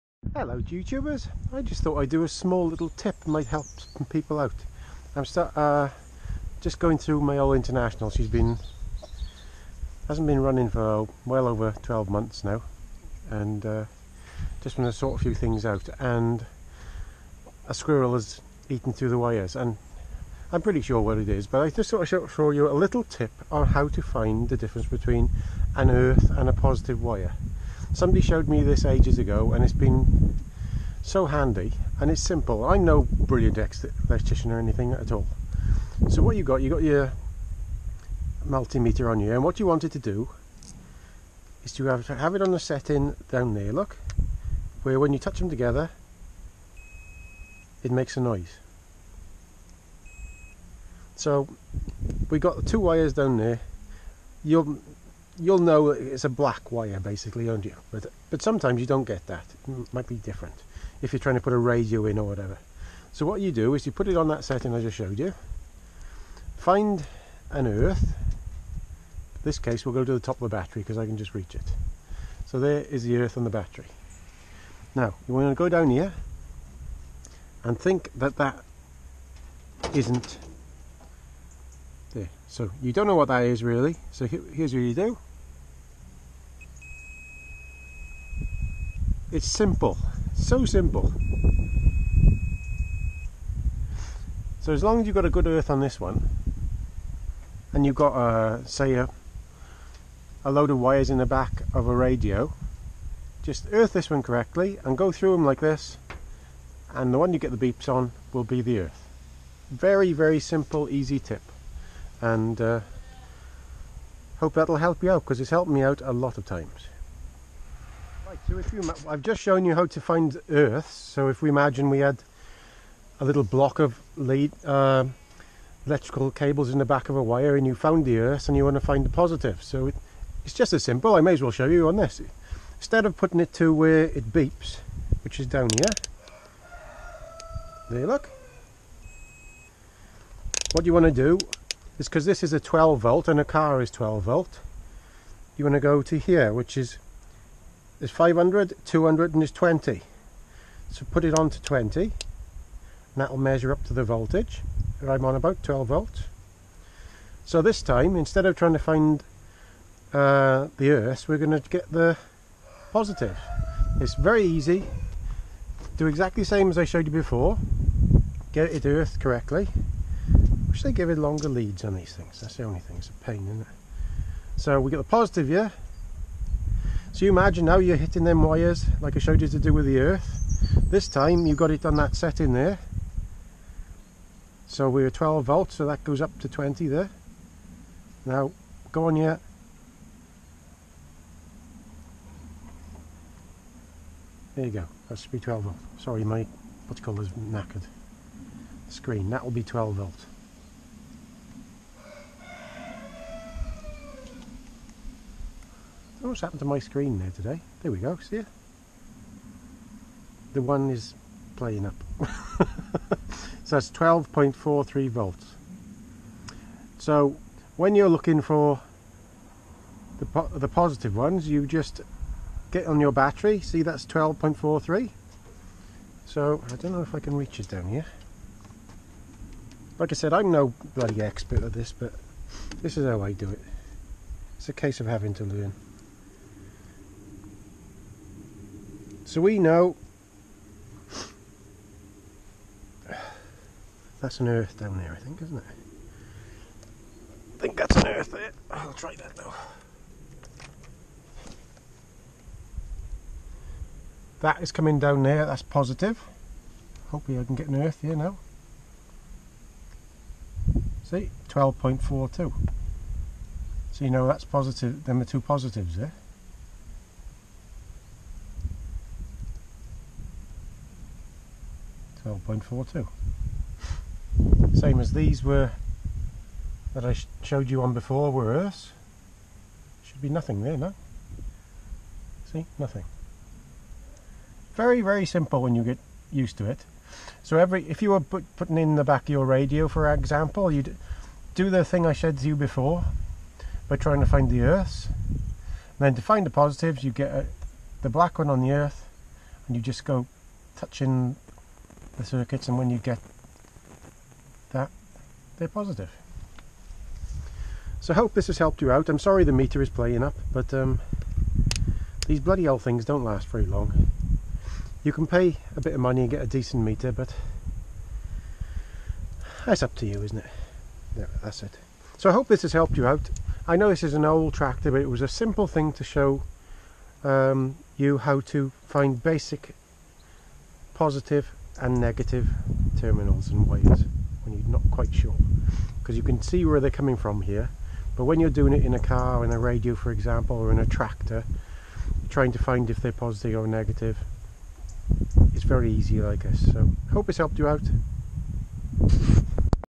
Really Simple 12 volt positive v negative multimeter test
This is a simple way to find negative wires and positive wires on a 12 volt system